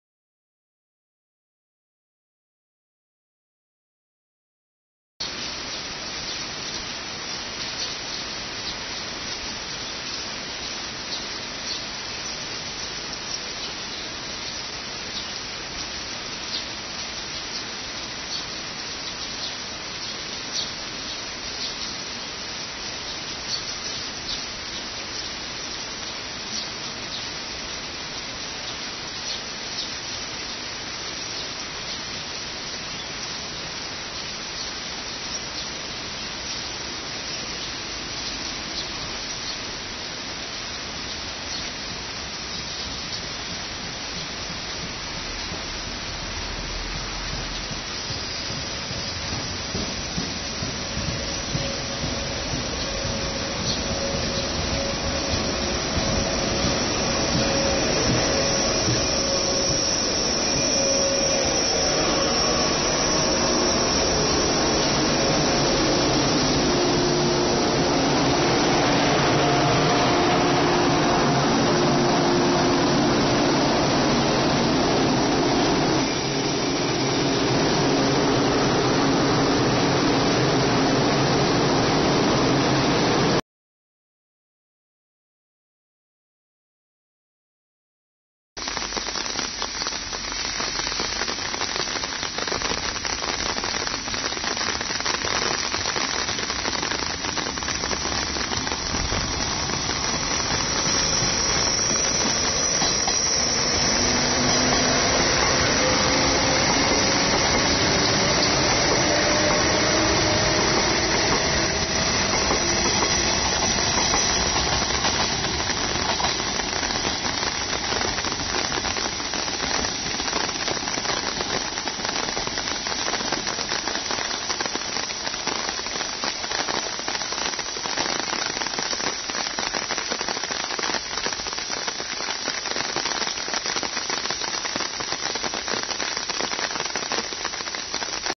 【ｽﾏﾎ撮影】 ニューなのはな ＠石越駅 2013.07.08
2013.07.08 午後２時２５分頃、石越駅に到着したのは団体臨時列車｢ニューなのはな｣です。約１９分停車後出発していきました。石越駅でなかなか見れる車両では無いので貴重な映像ではないでしょうか。